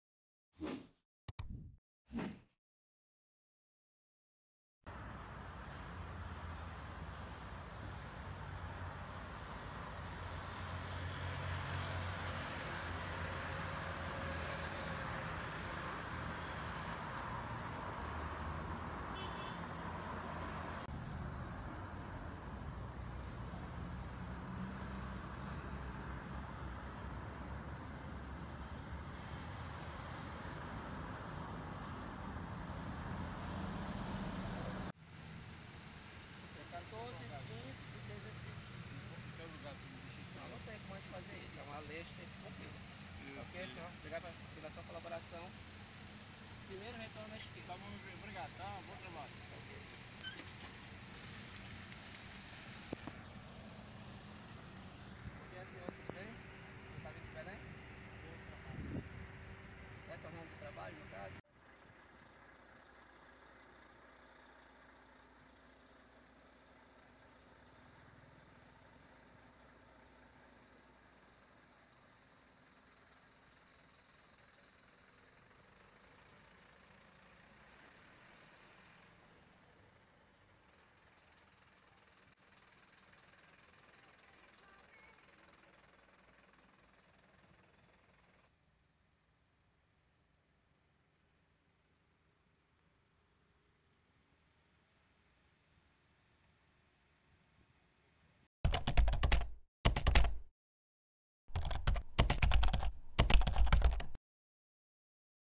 Fiscalização contra viagens proibidas no feriado engarrafa BR-316
Fluxo intenso a partir de Benevides causou transtornos nesta Sexta-Feira Santa. Muitos tentavam viajar a balneários, apesar da proibição estadual para conter pandemia do coronavírus